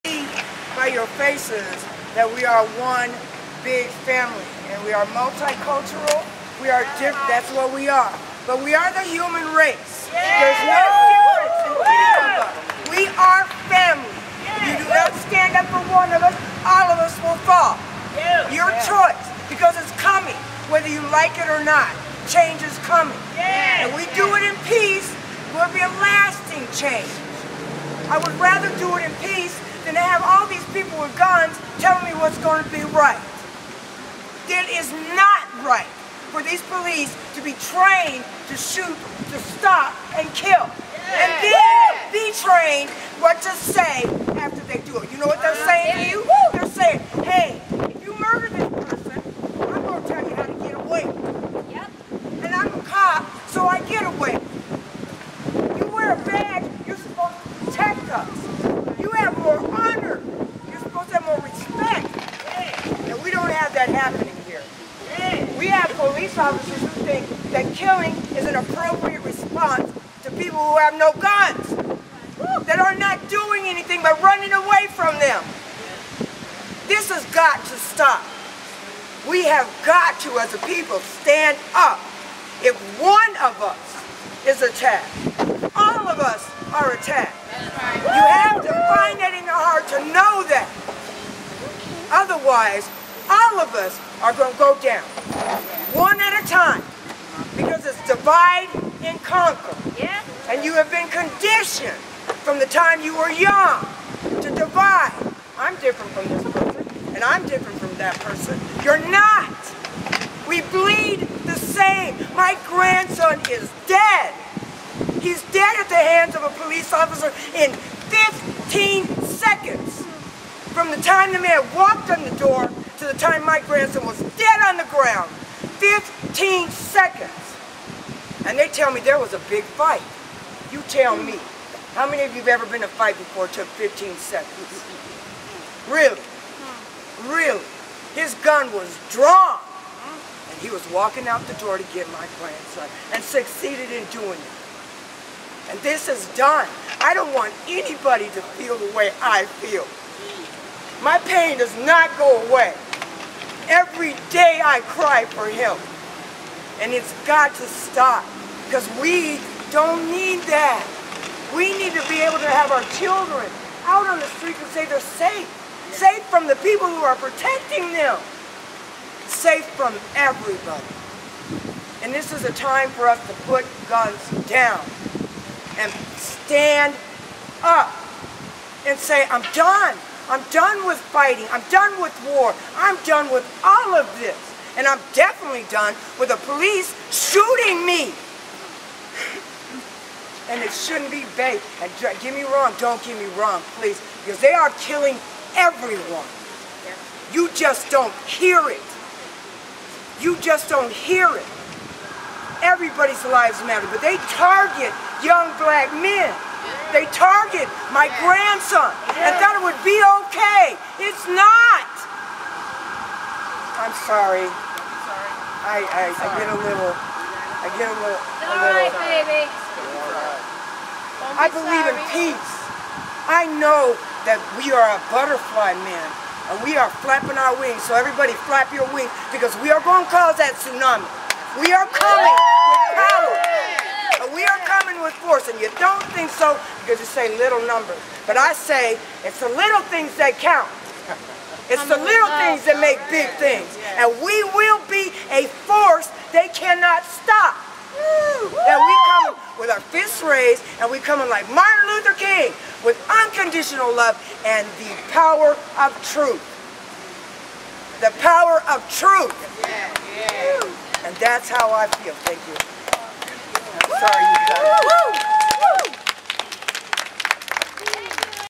Sharon Irwin-MillionMomsMarch-Rochester May 10 2015
TonyRobinson's grandmother Sharon Irwin speaking at the MillionMomsMarch-Rochester May 10 2015.